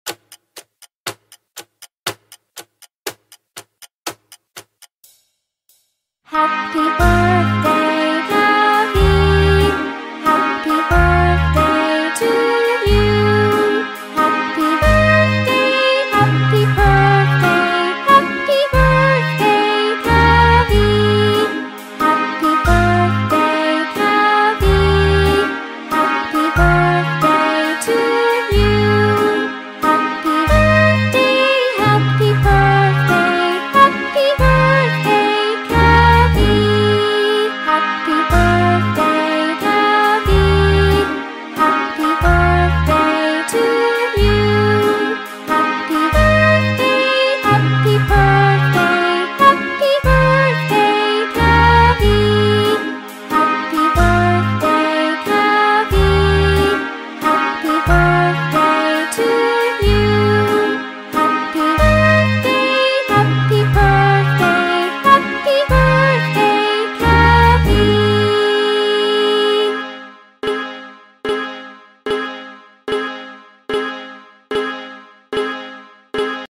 Happy Birthday Cathy | A special song just for you
Happy birthday, Cathy! We've written a special song just for you to celebrate your special day. This custom happy birthday song is dedicated to you. From all your friends and family, we wish you a very happy birthday, dear Cathy!

• happy birthday
• happy birthday song
• happy birthday Cathy
• happy birthday Cathy song
• Cathy happy birthday
• Cathy happy birthday song